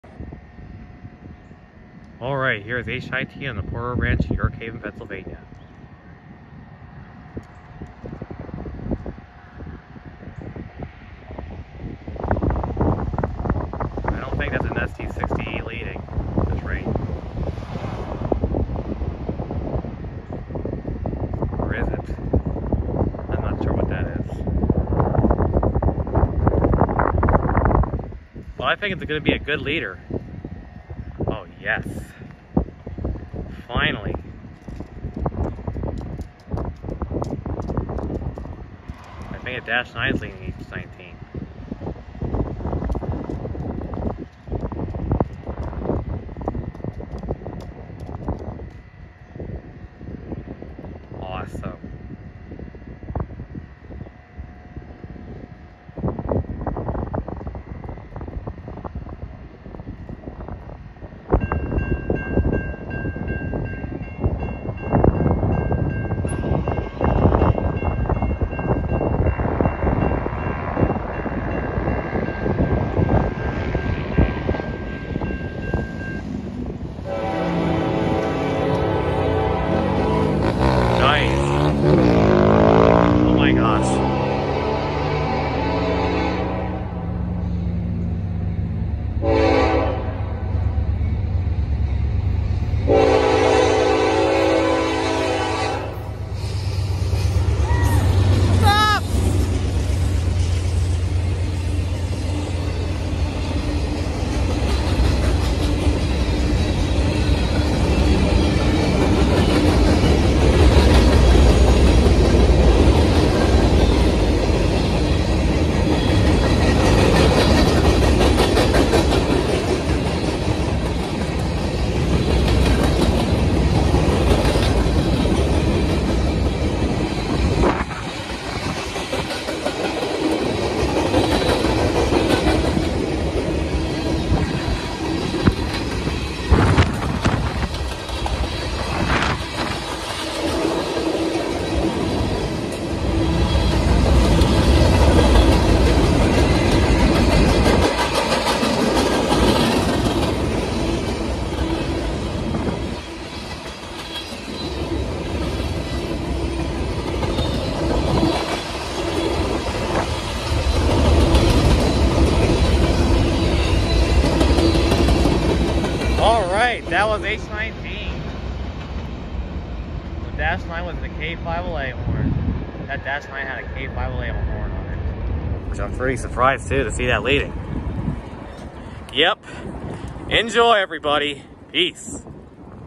Norfolk Southern H19 - 3/26/21
Norfolk Southern local freight train H19 is seen crossing Locust Street on the Port Road Branch with a Nathan K5LLA horn in York Haven, Pennsylvania at 5:42 PM on Friday, March 26, 2021. The engines in this video are: NS 9397 (D9-44CW), NS 9478 (D9-44CW) and NS 5816 (GP38-3). Enjoy!